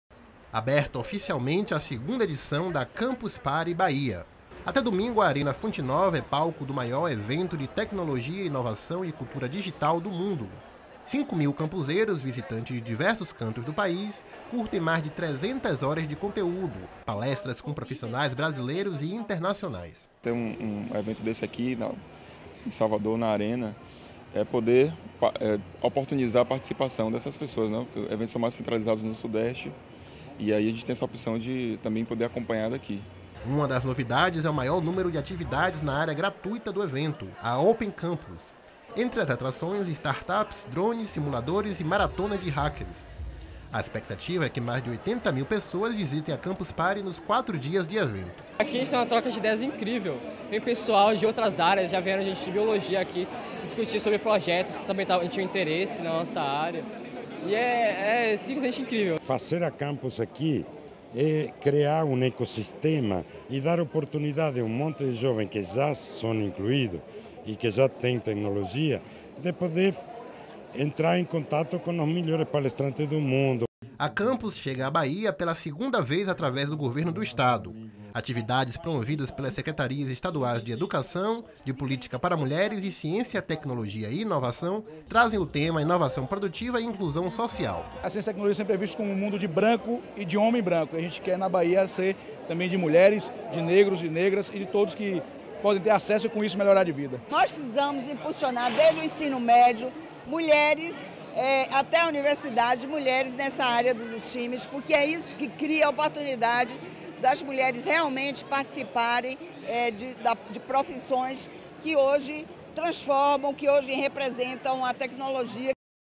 ABERTURA CAMPUS PARTY
Campus Party Bahia começa nesta quinta. Maior experiência tecnológica do mundo recebe mais de 80 mil visitantes e participantes até o fim de semana na Arena Fonte Nova